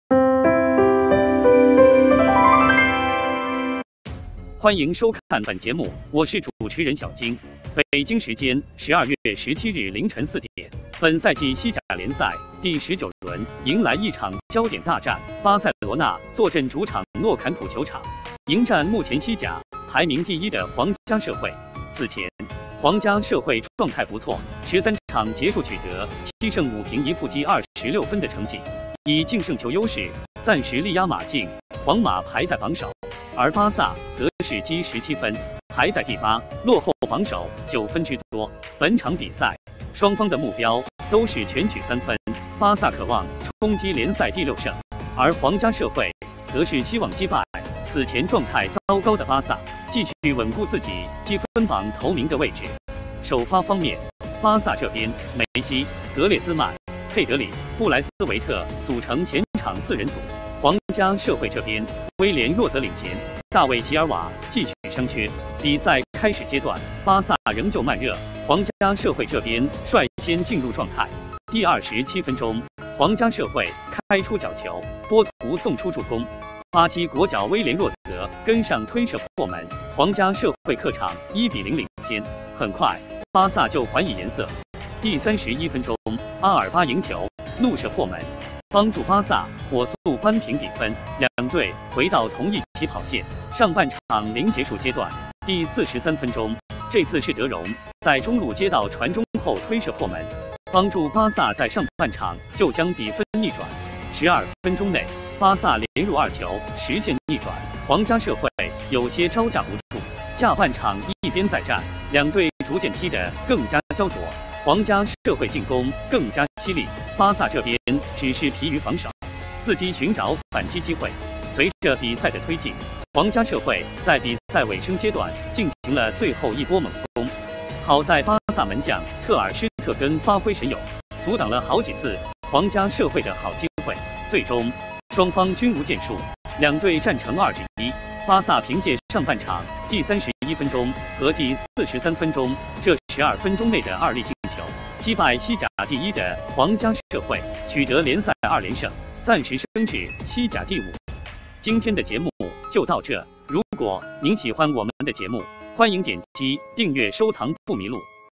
2:1！“巴萨”12分钟2球逆转：击倒西甲第一名，连赢2场并飙升至3名
北京时间12月17日凌晨4点，本赛季西甲联赛第19轮迎来一场焦点大战：巴塞罗那坐镇主场诺坎普球场，迎战目前西甲排名第一的皇家社会。此前，皇家社会状态不错，13场结束取得7胜5平1负积26分的成绩，以净胜球优势暂时力压马竞、皇马排在榜首。